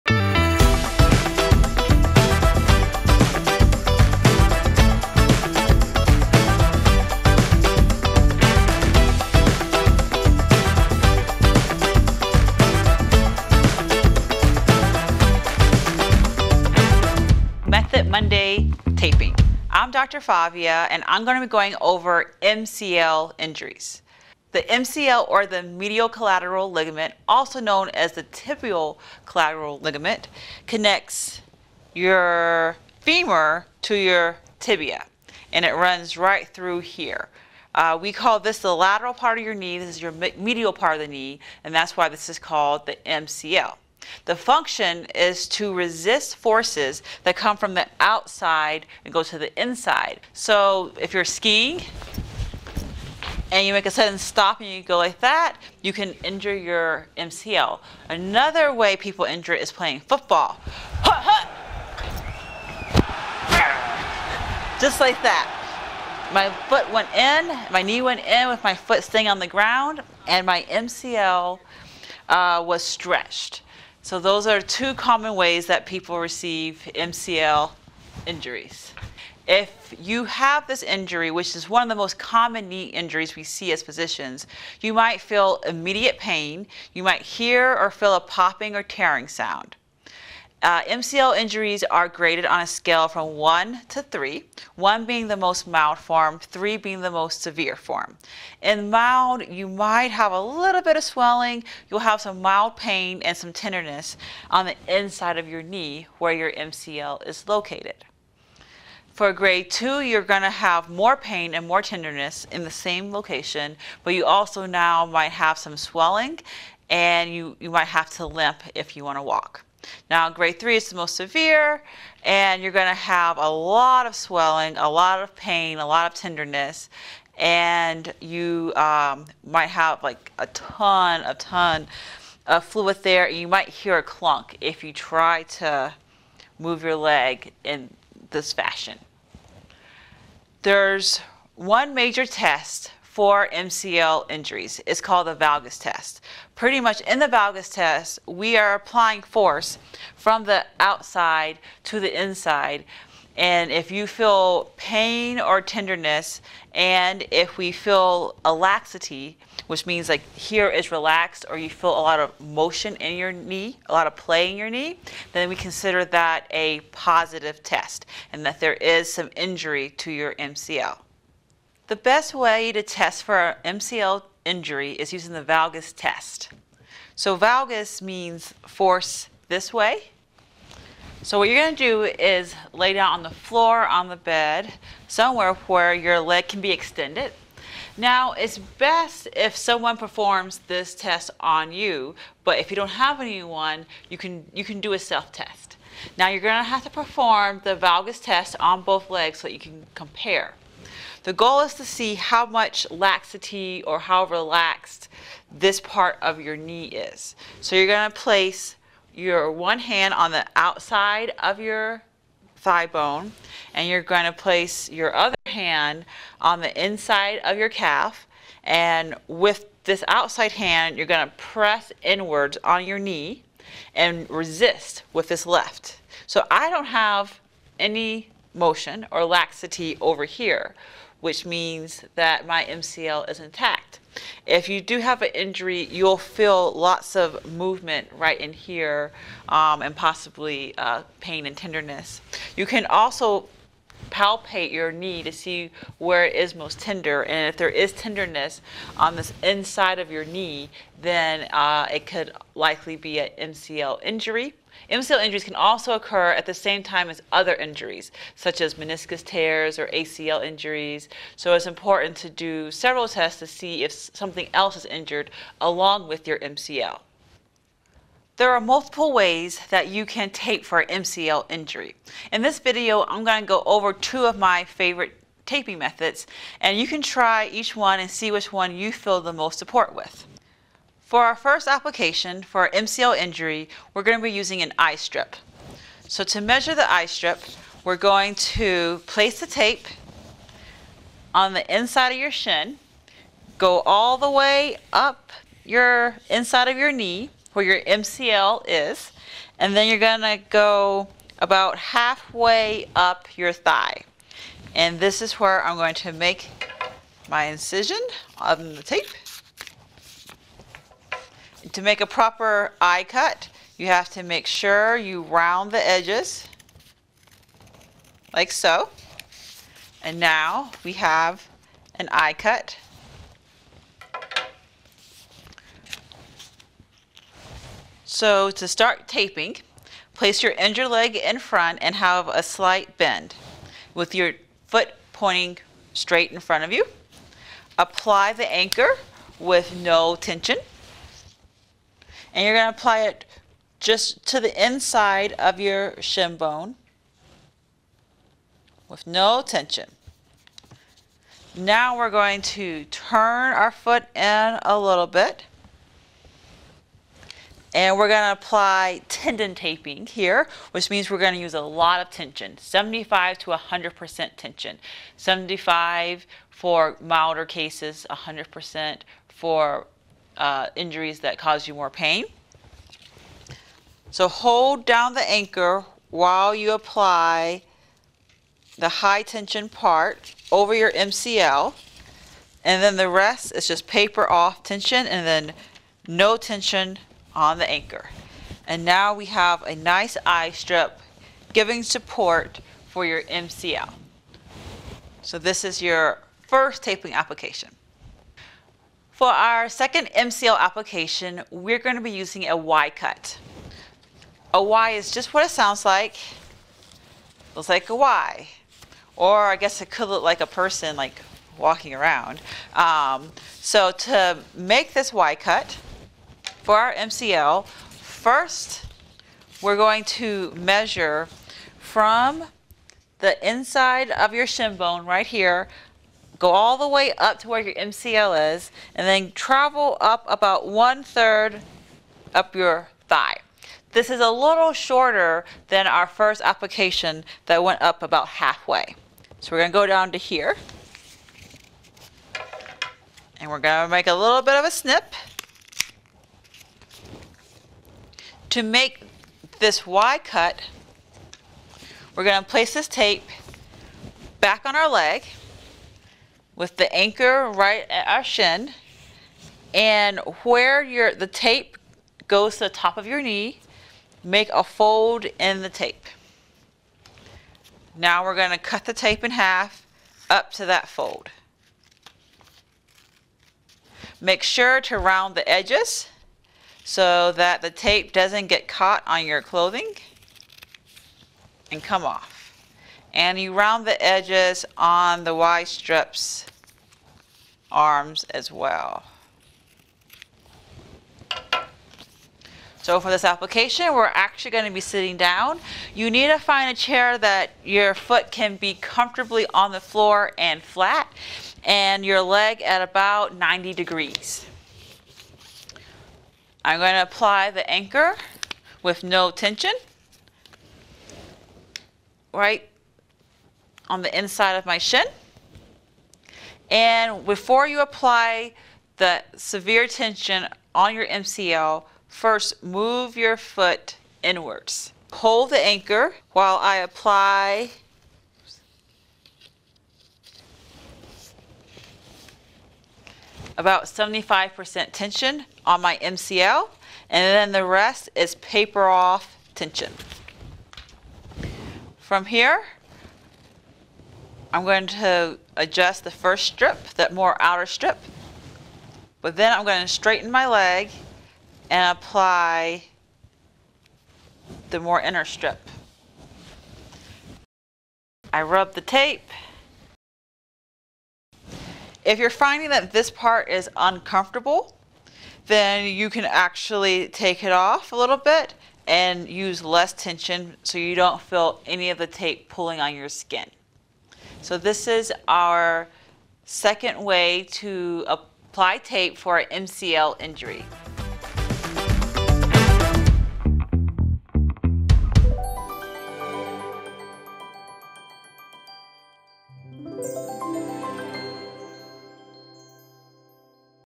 MCL - Kinesio Taping Application Instructions
Dr. Favia Dubyk teaches how to do Kinesio Taping for the MCL.